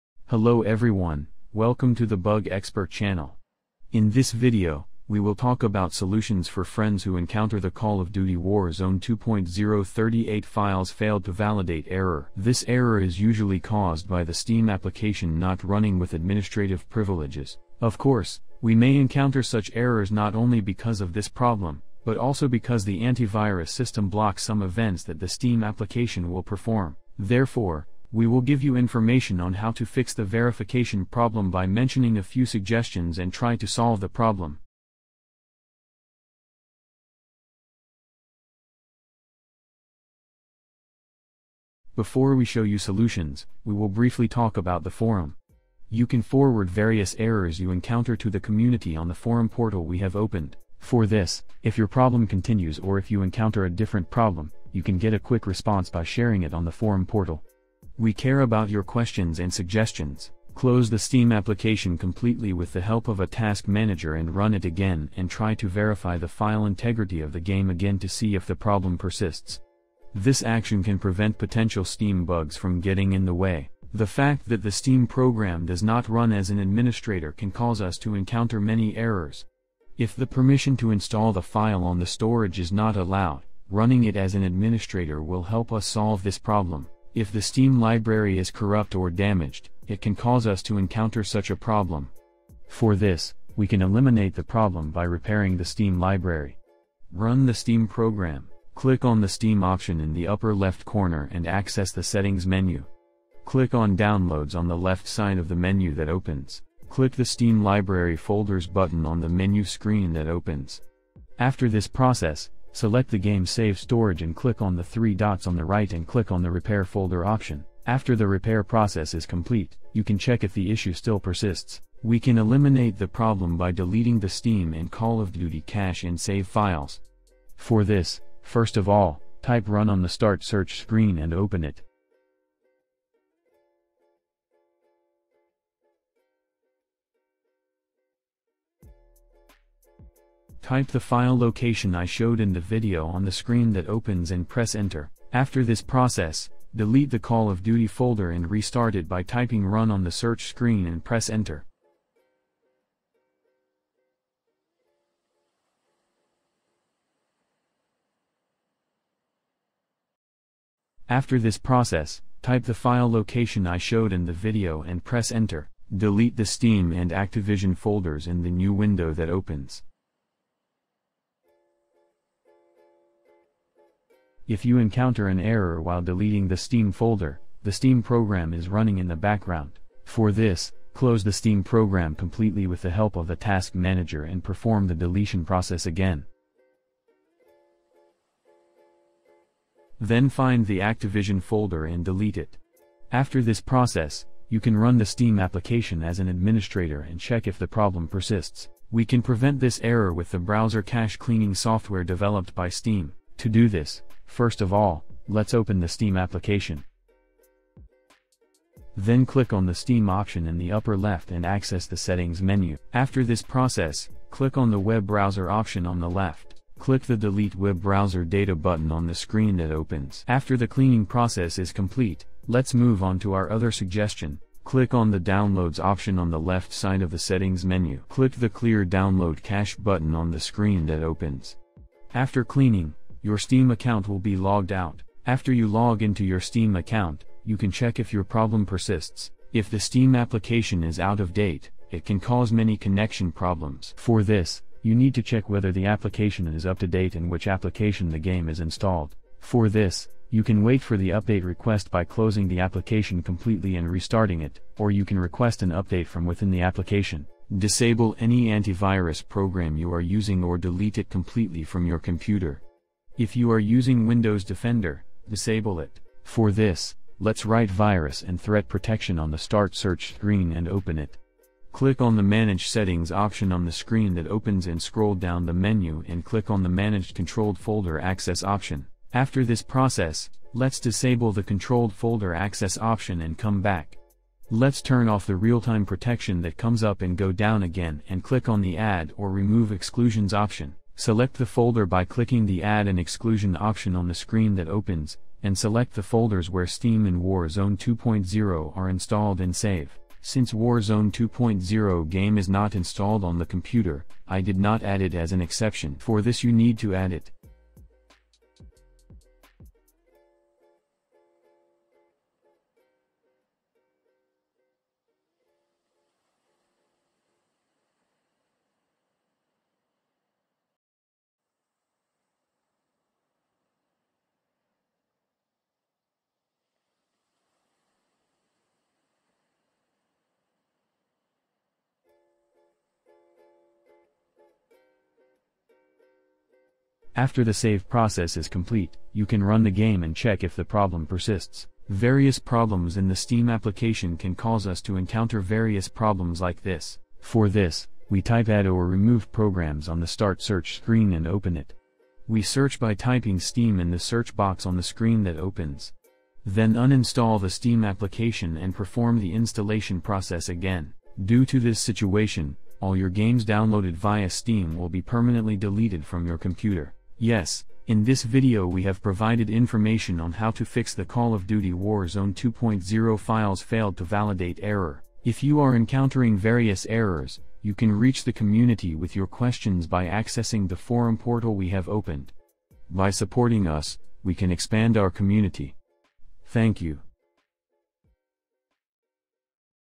Fix Call of Duty Warzone 2 0 38 Files Failed To Validate Error
In this video, we will try to resolve the "38 Files Failed To Validate and Will Be Reacquired" error that Call of Duty Warzone 2.0 players encounter when they want to verify their game files on Steam.